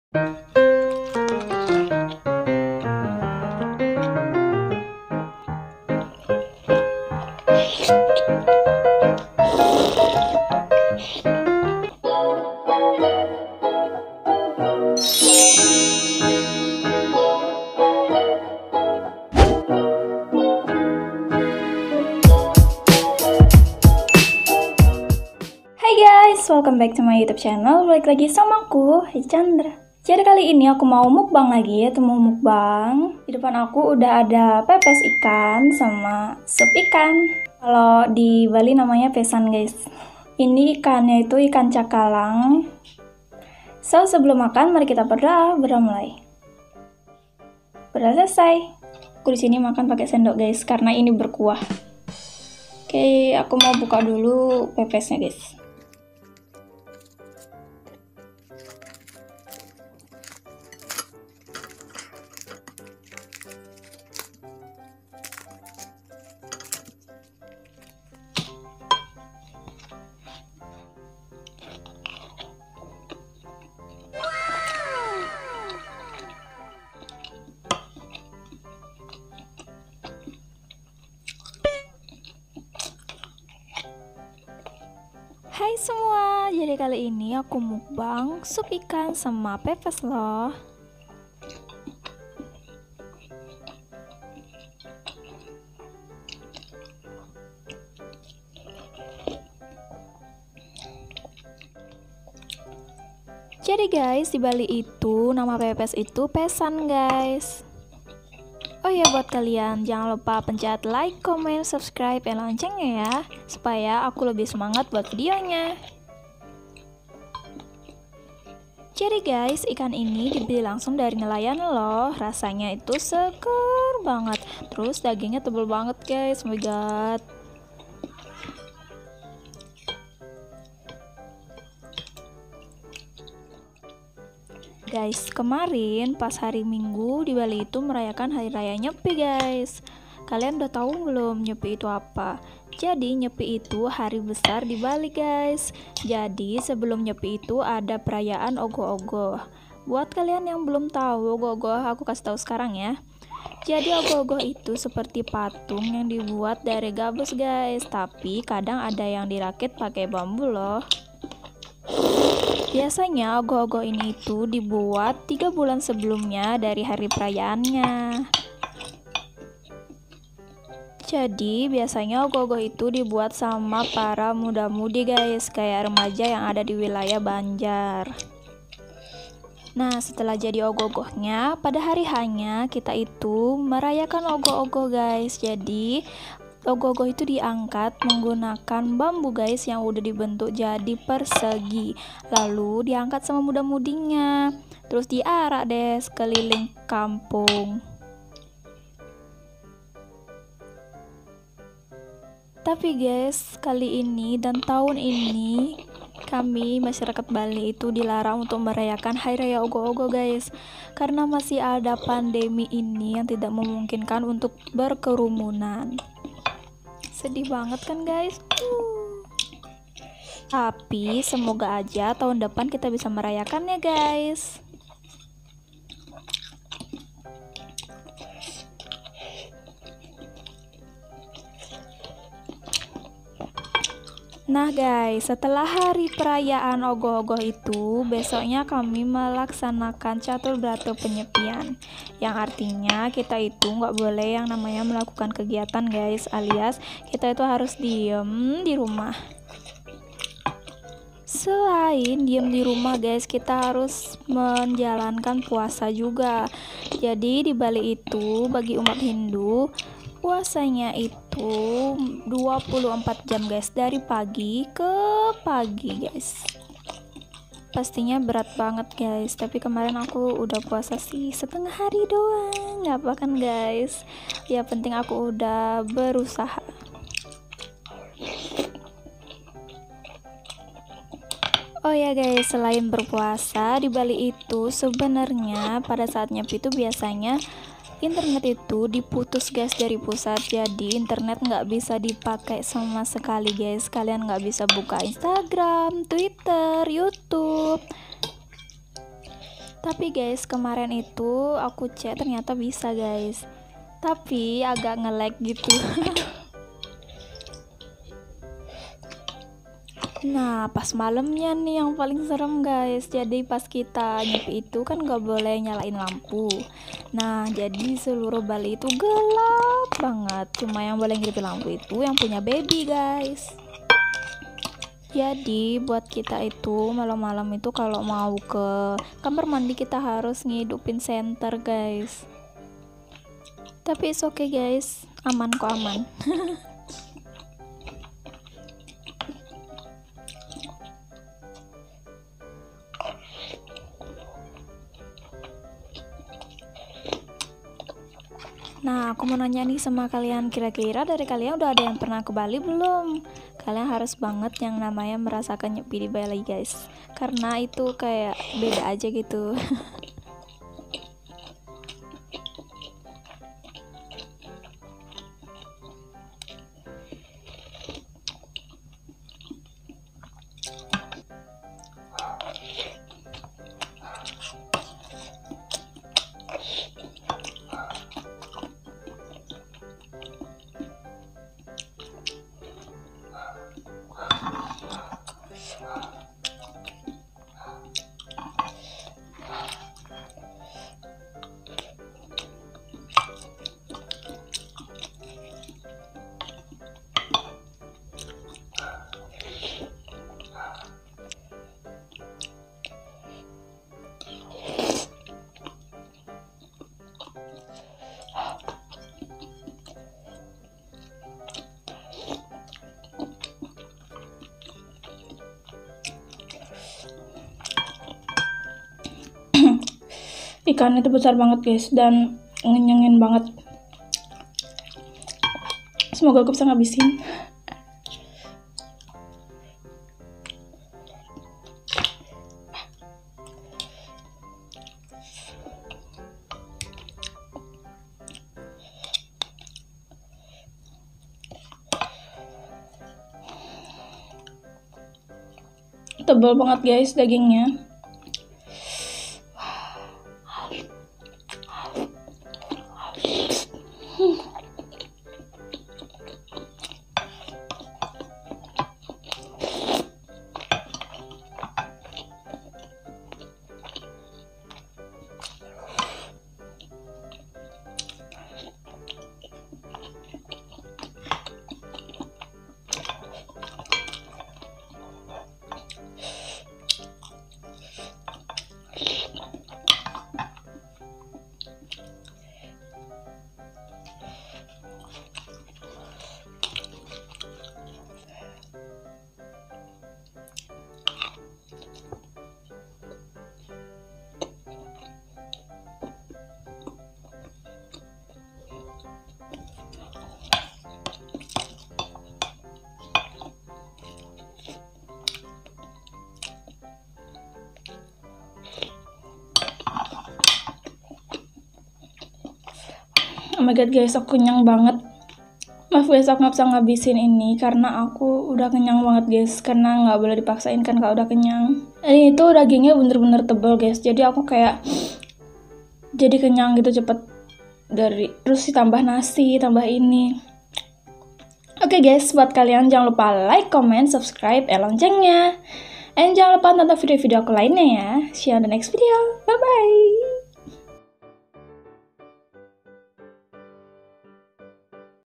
MUKBANG SOP IKAN DAN PEPES IKAN PEDAS!!! LANGSUNG DARI NELAYANNYA!!
MUKBANG SOP IKAN DAN PEPWS IKAN GUYSSS! PEDES BANGETTTTT

Hai balik lagi sama aku hey chandraa✨

Mukbang (먹방) berasal dari kata “meoknun” (먹는) dan “bangsong” (방송) yang memiliki arti makan dan broadcast (siaran). Singkatnya, mukbang adalah tayangan seseorang yang sedang menyantap makanan dan biasanya disiarkan secara live. Atau bisa kita singkat lagi jadi makan online.

Maaf kalau ada salah" kata😉

Terimakasi sudah menonton ✨